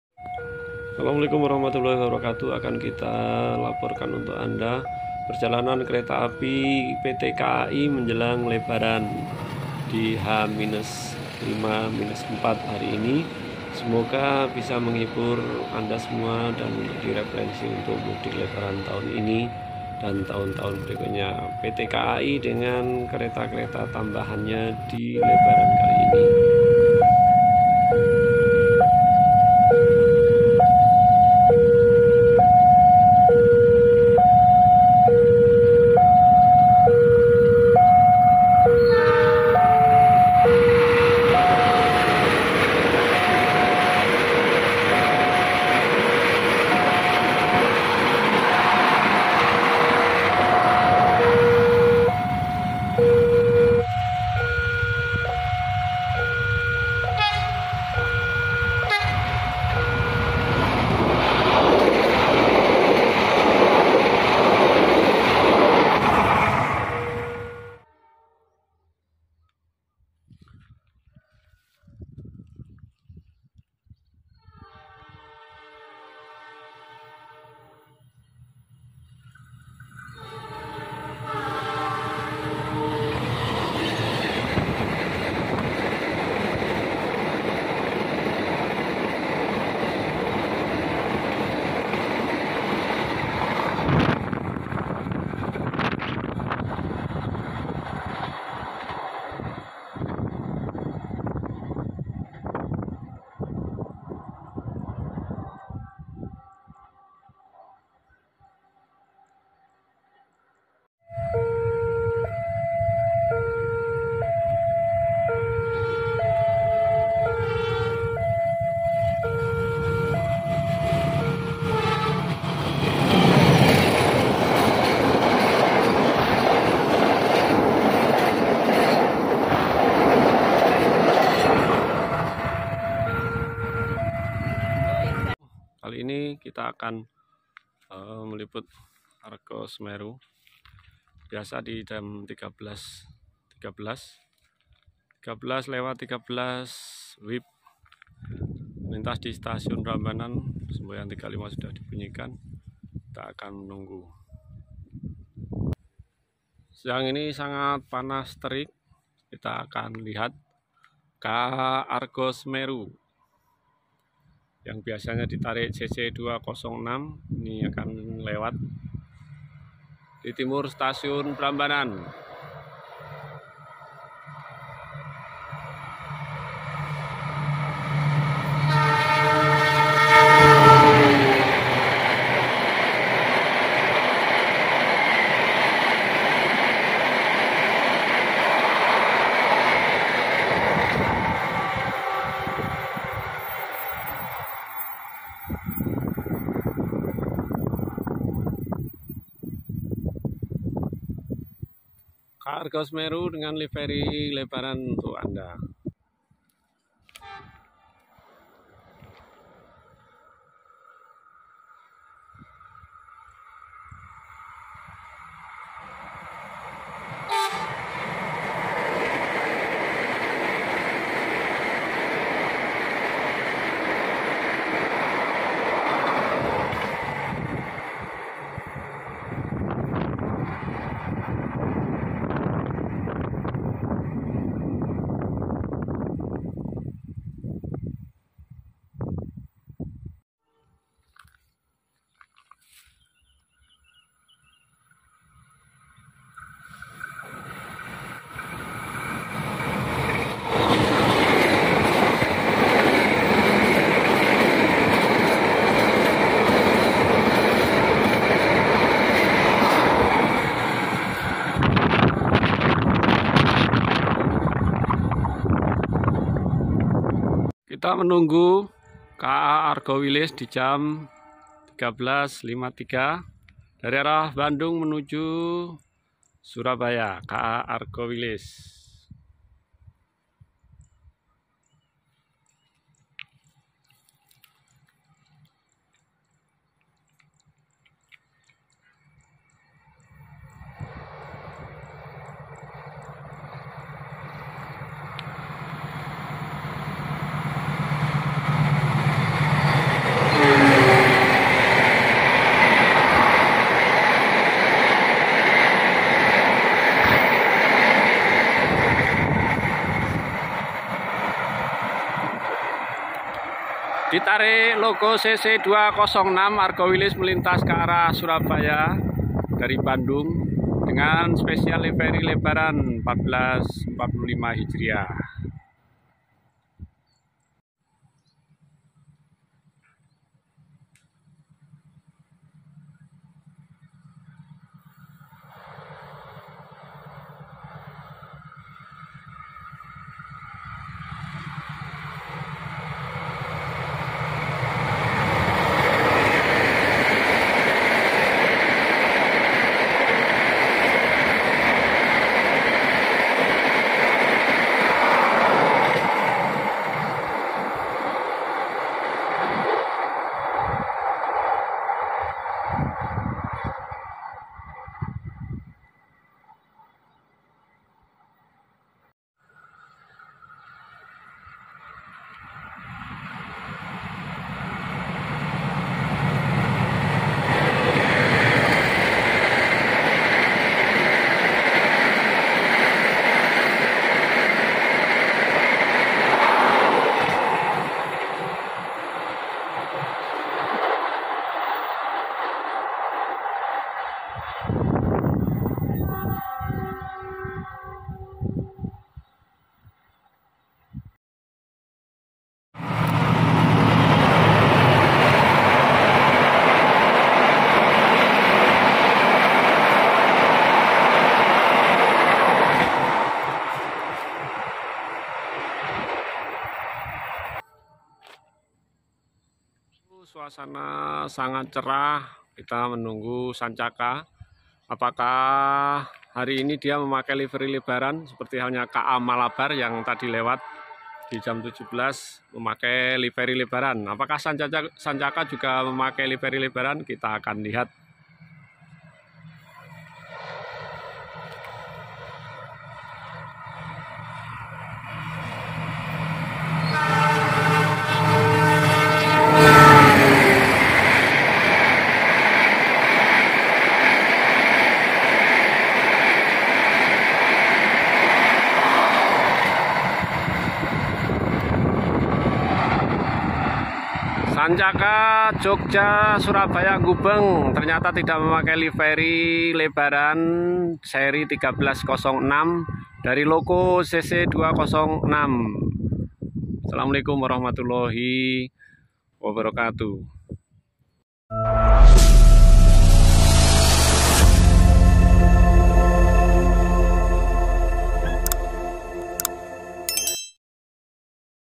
Kesibukan jalur kereta api menjelang lebaran 1445 H
Kesibukan jalur kereta api menjelng lebaran 1445 H